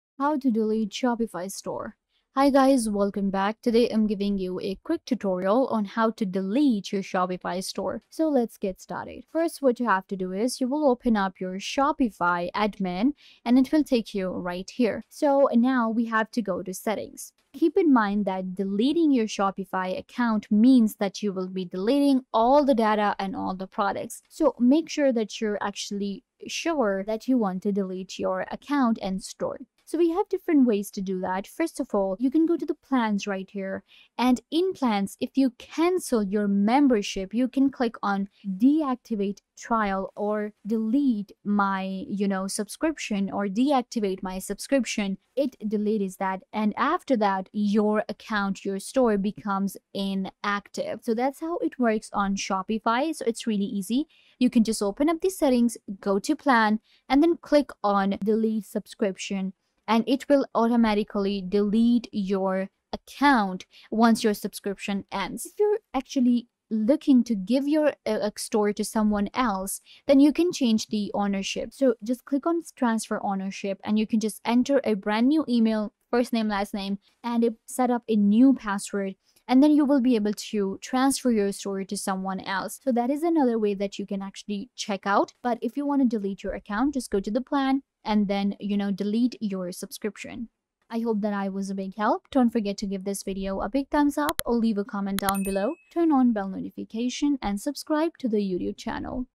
How To Delete Shopify Store 2026 (BEST WAY)
In this video, I show you step-by-step how to delete your Shopify store. It’s really easy, and you can learn to do it in just a few minutes by following this super helpful tutorial to get a better understanding of everything.